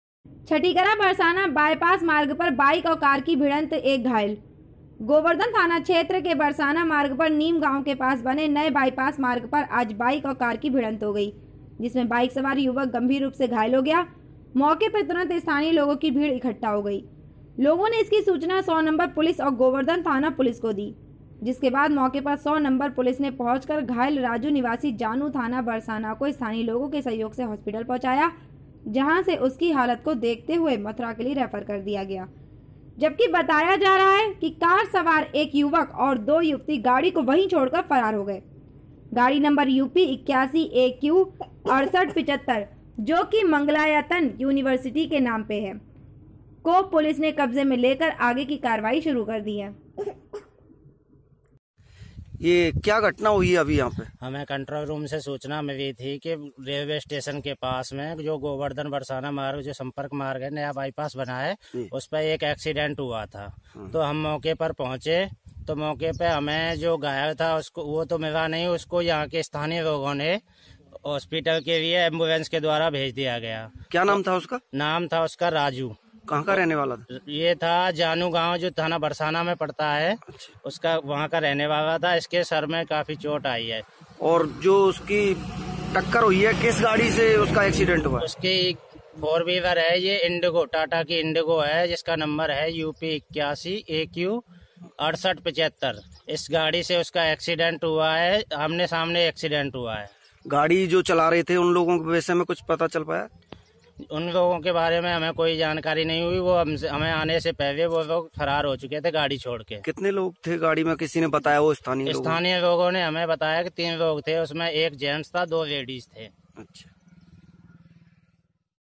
Mathura छटीकरा बरसाना बायपास मार्ग पर बाइक और कार की भिड़ंत एक घायल
छटीकरा बरसाना बायपास मार्ग पर बाइक और कार की भिड़ंत एक घायल

गोवर्धन थाना क्षेत्र के बरसाना मार्ग पर नीम गांव के पास बने नए बाईपास मार्ग पर आज बाइक और कार की भिड़ंत हो गई जिसमें बाइक सवार युवक गंभीर रूप से घायल हो गया । मोके पे तुरंत स्थानीय लोगों की भीड़ इकट्ठा हो गई  लोगों ने इसकी सूचना 100 नंबर पुलिस और गोवर्धन थाना पुलिस को दी जिसके बाद मौके पर 100 नंबर पुलिस ने पहुंचकर घायल राजू निवासी जानू थाना बरसाना को स्थानीय लोगों के सहयोग से हॉस्पिटल पहुंचाया जहां से उसकी हालत को देखते हुए मथुरा के लिए रेफर कर दिया गया जबकि बताया जा रहा है कि कार सवार एक युवक और दो युवती गाड़ी को वहीं छोड़कर फरार हो गए गाड़ी नंबर यूपी 81a q 6875 ( जो कि मंगलायतन यूनिवर्सिटी के नाम पे है ) को  पुलिस ने कब्जे में लेकर आगे की कार्यवाही शुरू कर दी है।

 100 पुलिसकर्मी